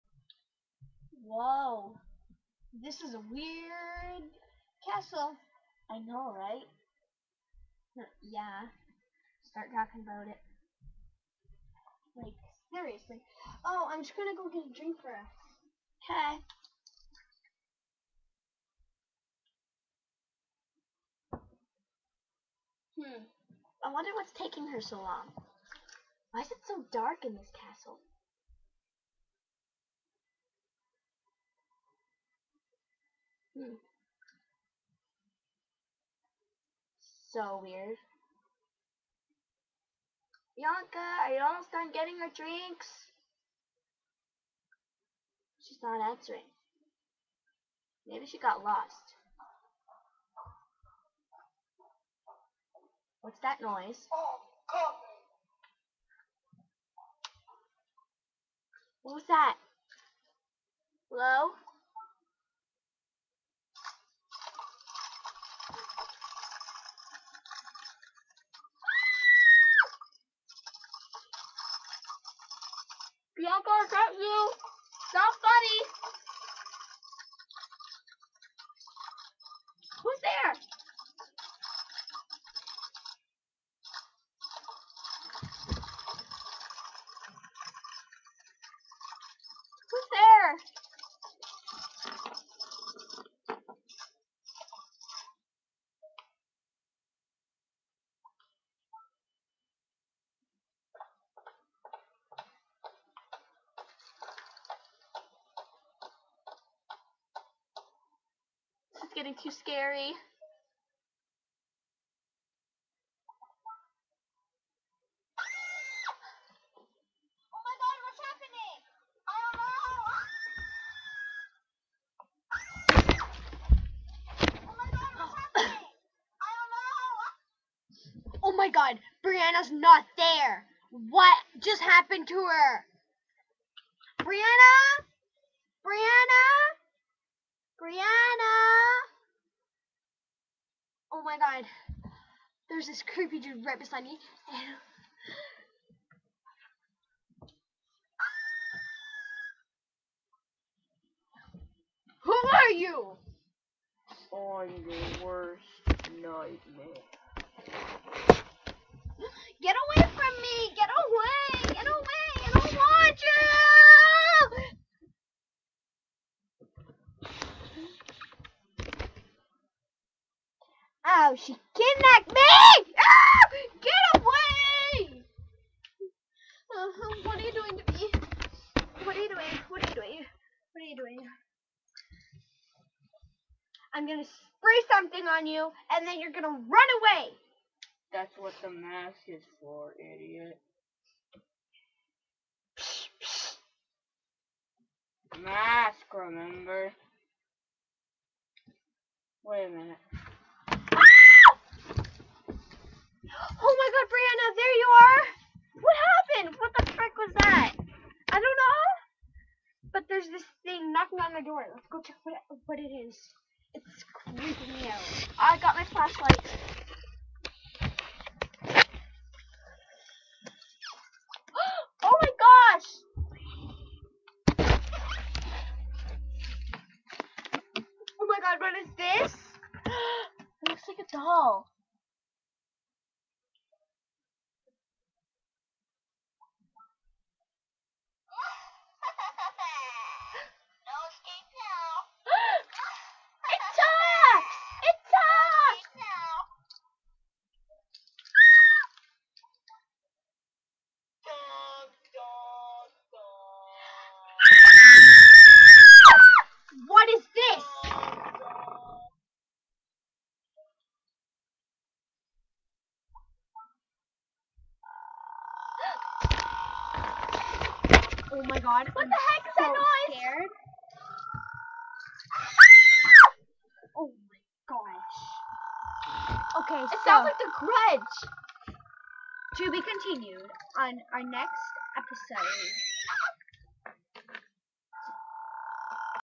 Haunting In Mansion #35 Ep.1 Weird Doll and Weird Noises
This new thriller series is about 2 girls go into a haunted mansion and experience super creepy stuff!!